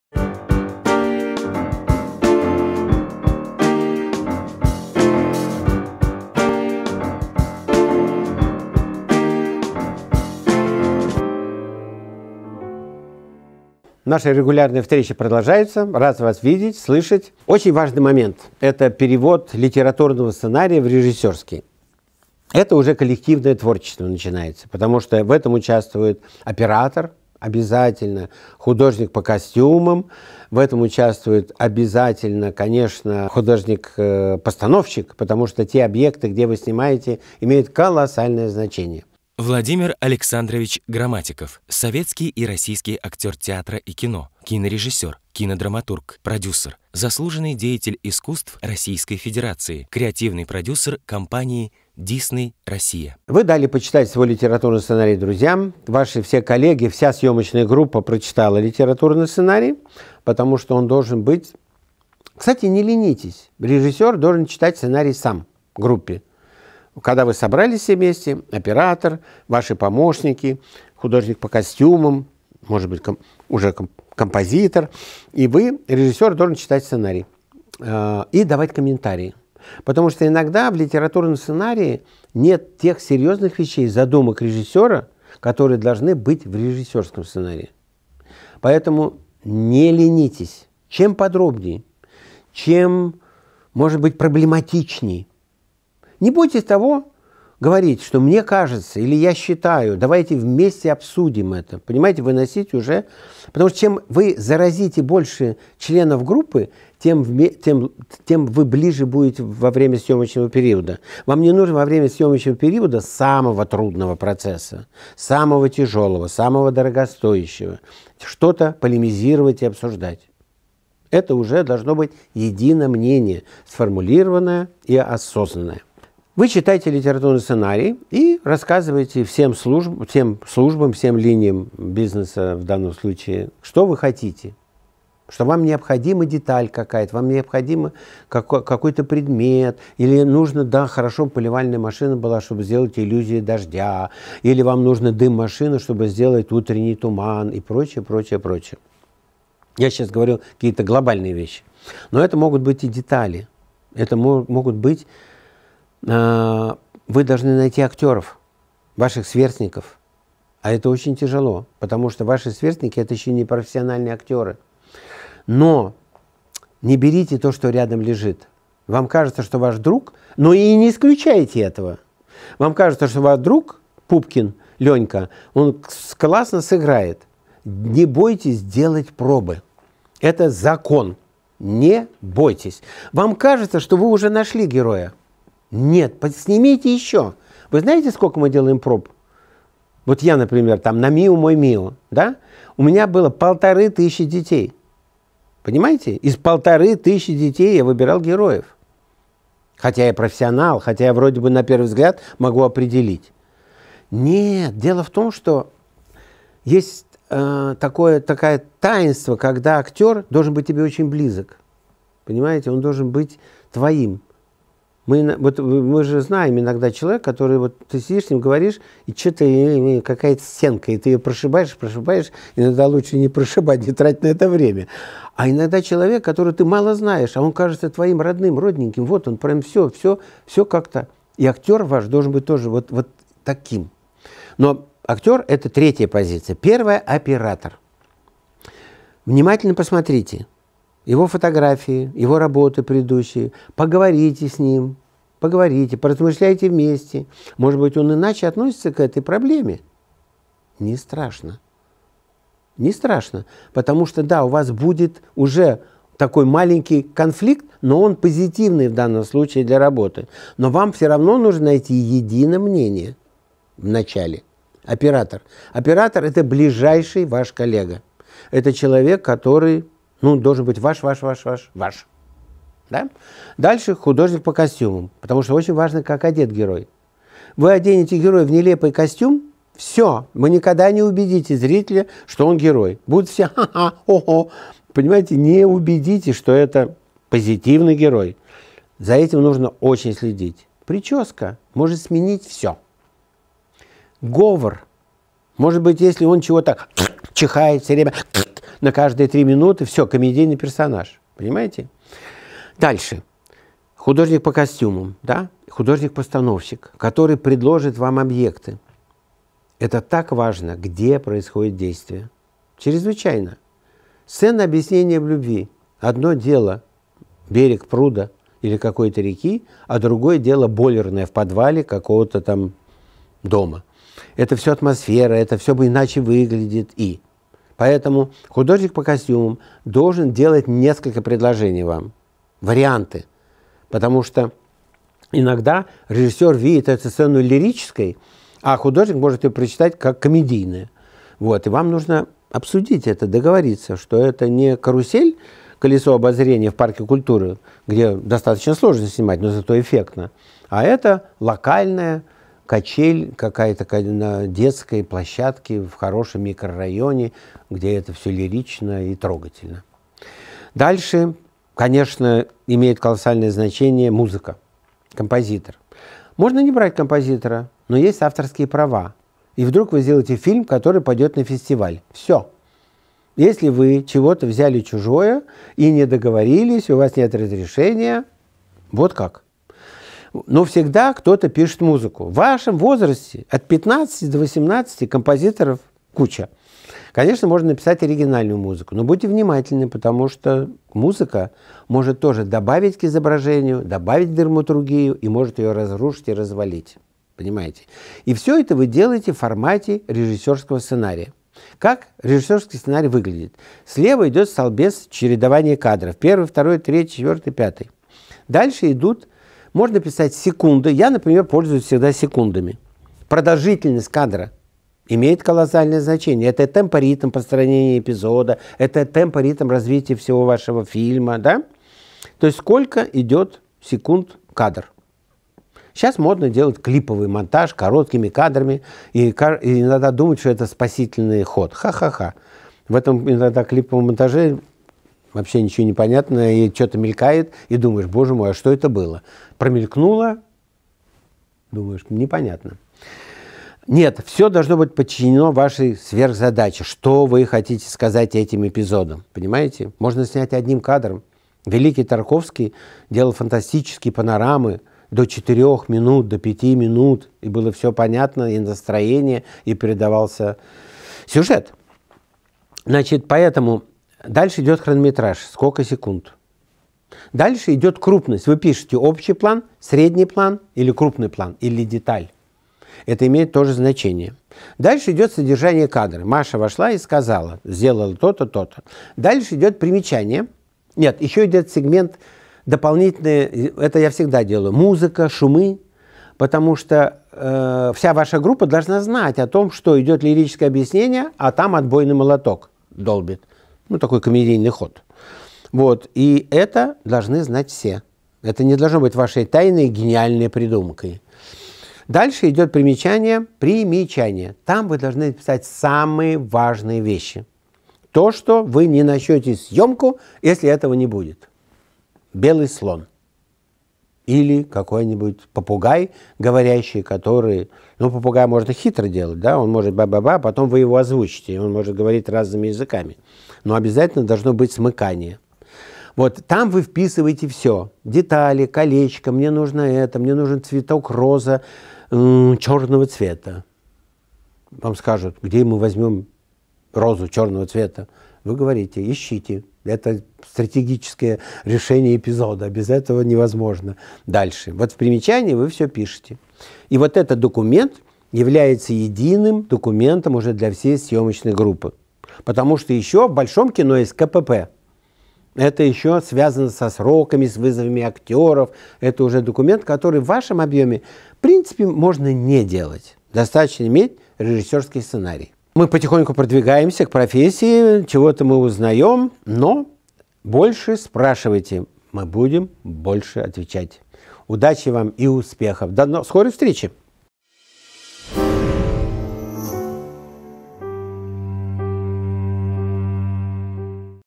Что такое режиссерский сценарий? Урок Владимира Грамматикова «Поколению М»
Специально для «Поколения М» режиссер Владимир Грамматиков расскажет, что такое режиссерский сценарий, кто из членов съемочной команды работает над ним и почему, в конце концов, план, составленный из множества столбцов, аббревиатур и цифр, помогает творить искусство. Смотри урок, загружай собственные короткометражки на конкурс и побеждай. Лучший участник поедет на форум «Бумеранг» снимать профессиональный фильм! Еще больше конкурсов — на сайте